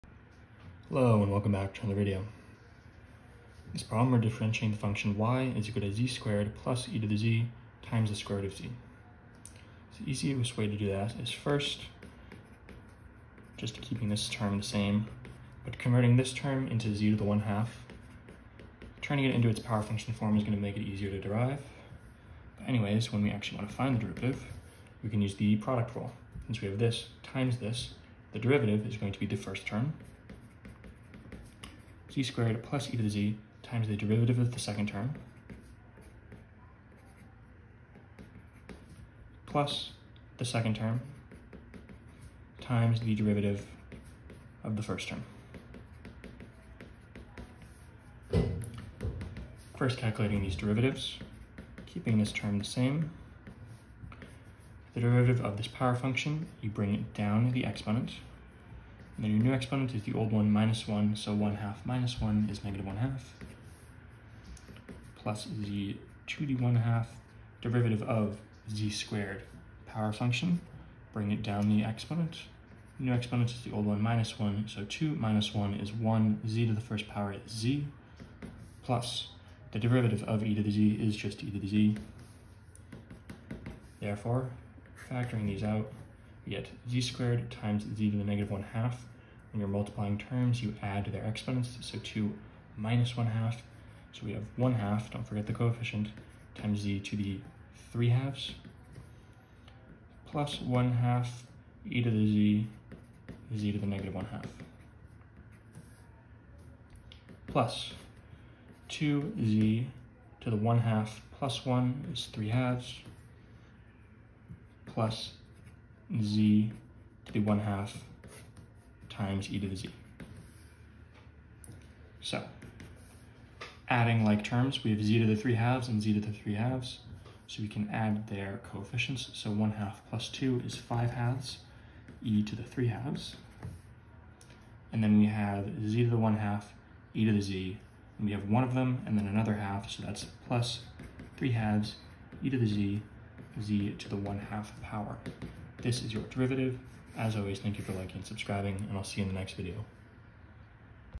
20. Differentiate. y=(z^2+e^z)√z
20. Differentiate.

y=(z^2+e^z)√z

Calculus: Early Transcendentals
Chapter 3: Differentiation Rules
Section 3.2: The Product and Quotient Rules
Problem 20